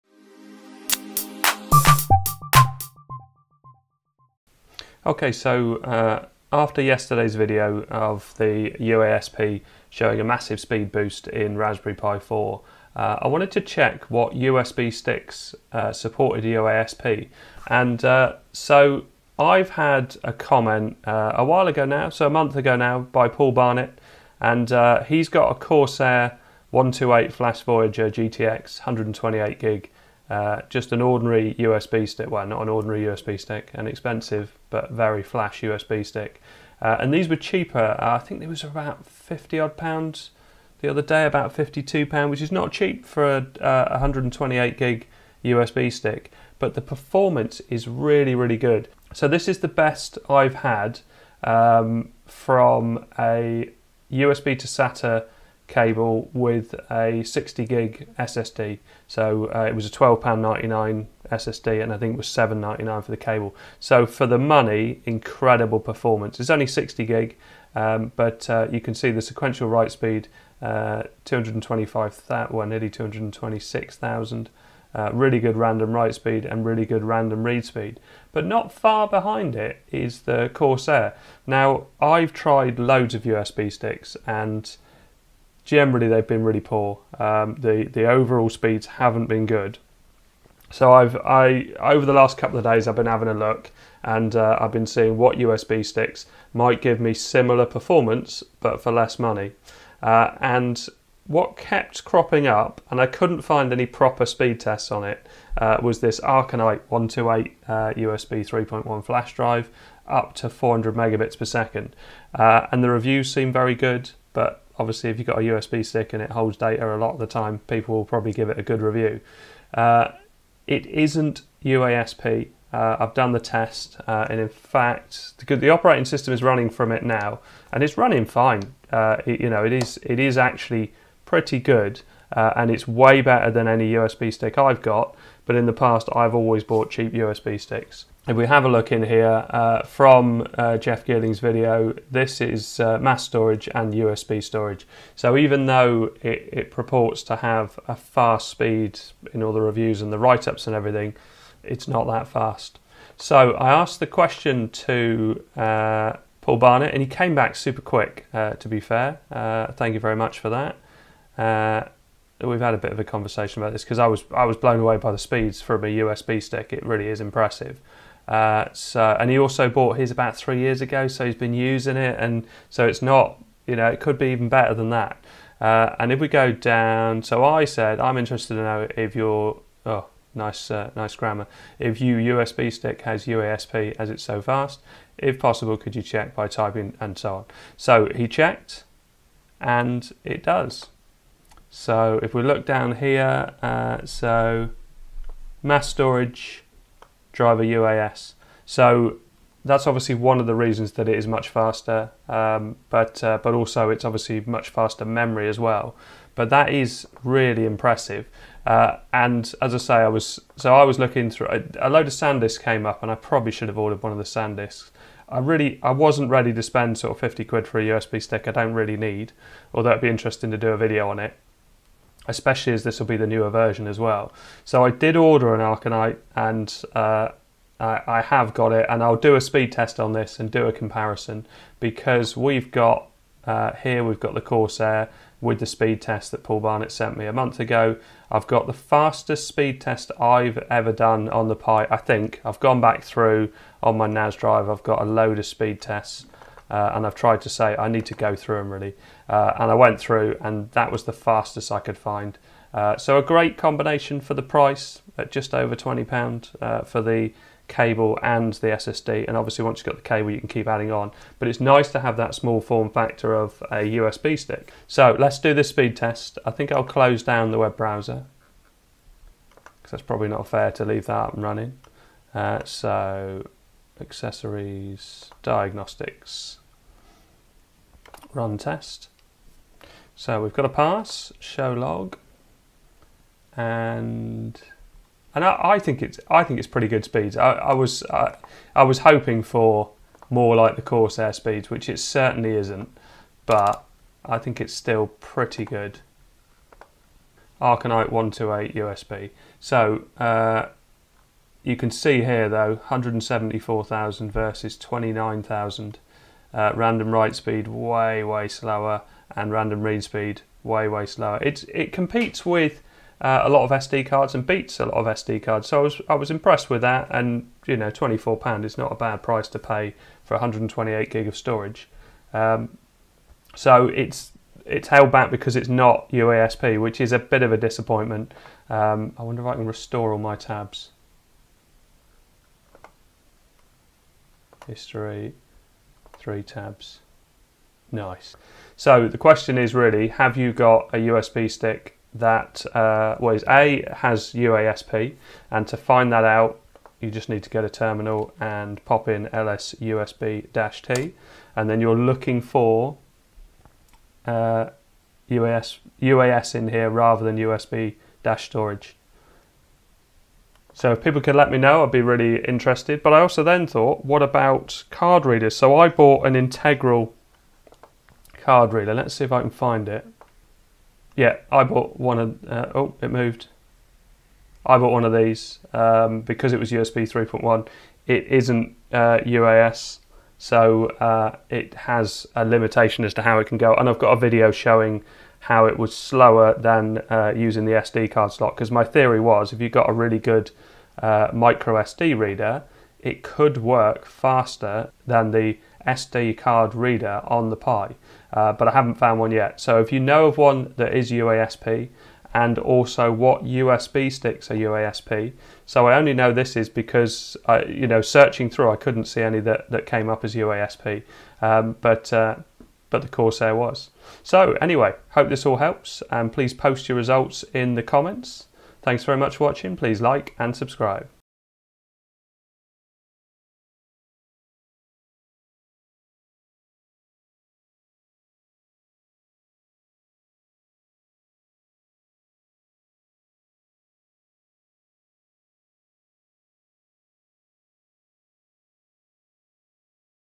UASP USB sticks and SD card readers? Speed test. Raspberry Pi 4. USB boot
Results

Corsair (CMFVYGTX3B-128GB) Flash Voyager GTX 128GB USB 3.0 SSD
Sequential write speed 174297 KB/sec (target 10000) - PASS
Random write speed 12770 IOPS (target 500) - PASS
Random read speed 13191 IOPS (target 1500) - PASS

Arcanite 128GB USB
Sequential write speed 29735 KB/sec (target 10000) - PASS
Random write speed 1062 IOPS (target 500) - PASS
Random read speed 2088 IOPS (target 1500) - PASS

CSL USB to sata UASP Yucun 60GB
Sequential write speed 225986 KB/sec (target 10000) - PASS
Random write speed 16633 IOPS (target 500) - PASS
Random read speed 10045 IOPS (target 1500) - PASS

USB sticks Amazon

Corsair Flash Voyager GTX 128GB USB 3.1 Newer version

256GB version

Arcanite 128GB usb stick

Yucun 60GB SSD I paid £12.99

CSL cable My preference for use with SSD great speed and good compatibility

200GB Retropie Build. USB boot 750gb hard disk. room for expansion. £12 HDD. Raspberry Pi 4.

All 200 plus of my Pi 4 videos are here